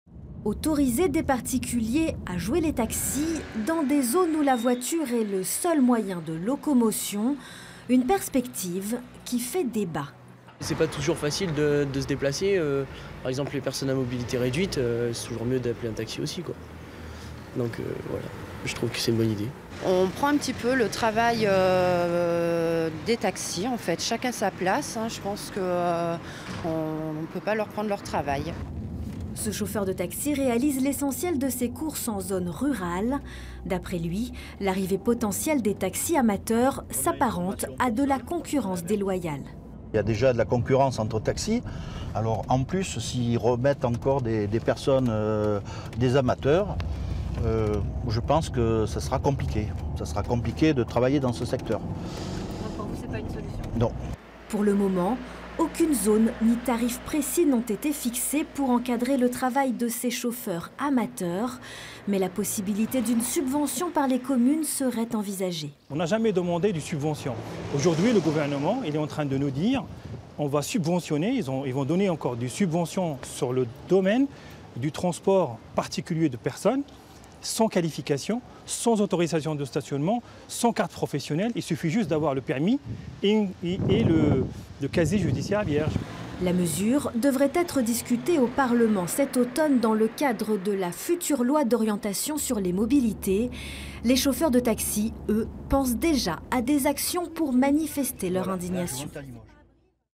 Taxis en zones rurales
En zones rurales, là où la voiture est souvent l'unique moyen de se déplacer, les particuliers pourraient prochainement être autorisés à devenir taxis. Une mesure envisagée par le Gouvernement et dont le Parlement devrait débattre à la rentrée, en septembre. Même s'il n'y a encore rien de concret, les taxis professionnels parlent d'ores et déjà de concurrence déloyale.
Le reportage à Larche, en Corrèze, de Caroline Huet et Marine Nadal. Montage : Nicolas Stil.
Intervenants : Jean-Luc Roche, artisan-taxi ; Ahmed Senbel, président de la Fédération des Taxis indépendants.